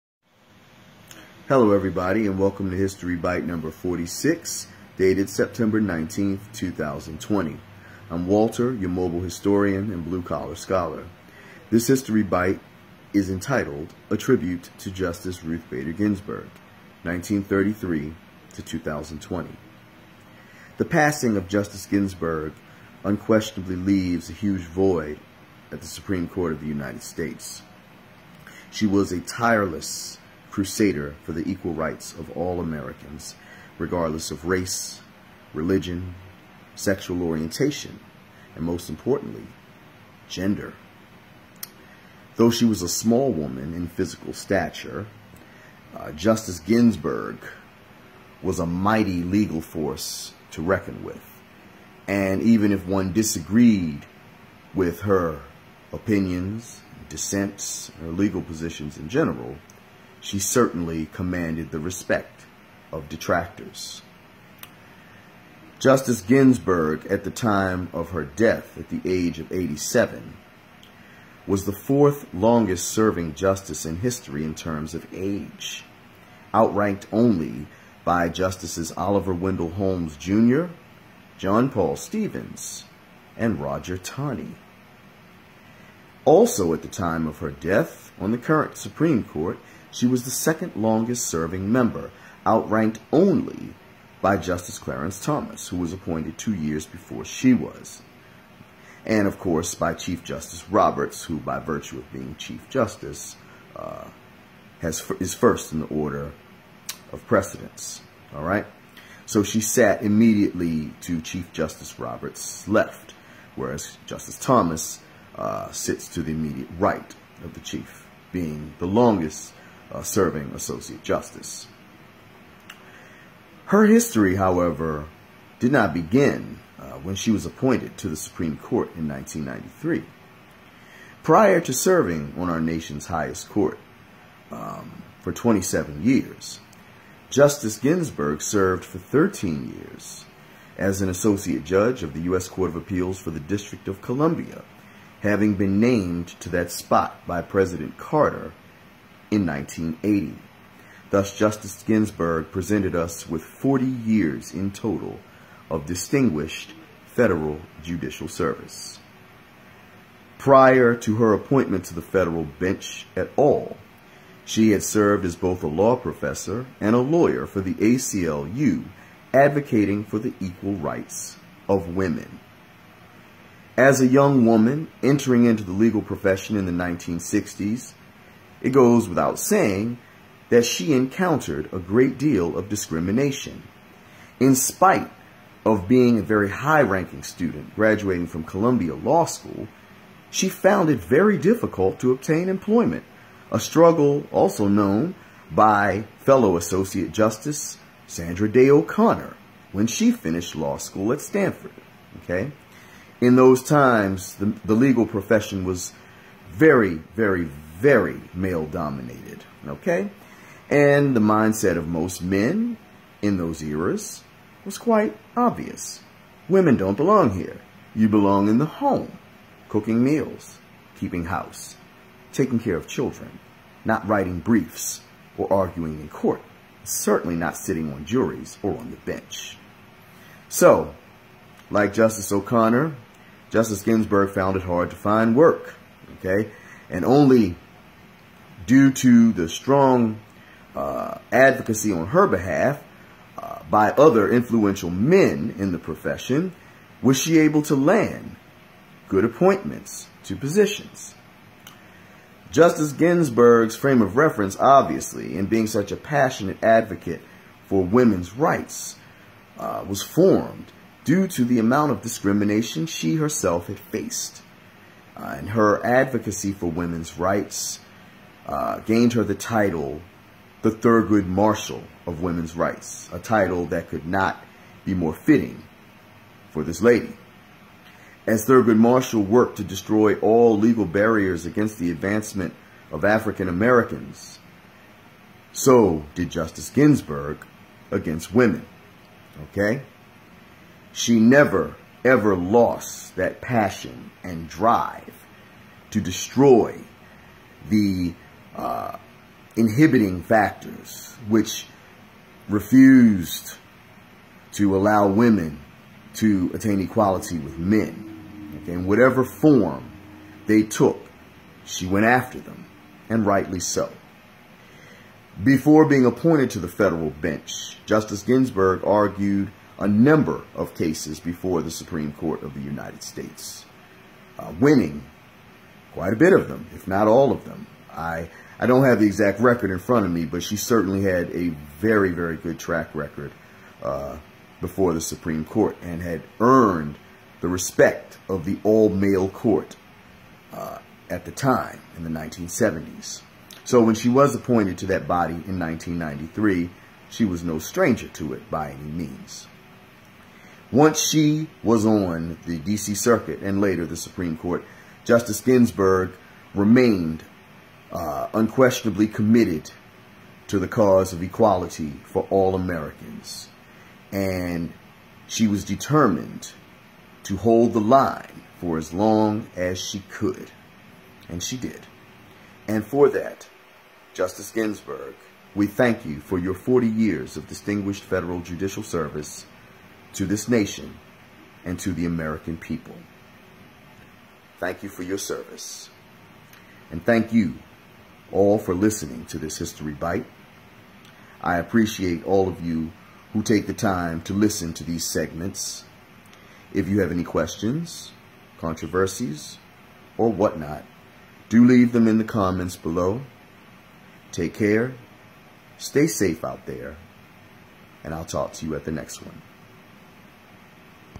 Tribute to Justice Ruth Bader Ginsburg (1933-2020)
This video is a biographical tribute to former U.S. Supreme Court justice Ruth Bader Ginsburg. Ginsburg served as an associate justice of the U.S. Supreme Court from  August 1993 until her death in September 2020.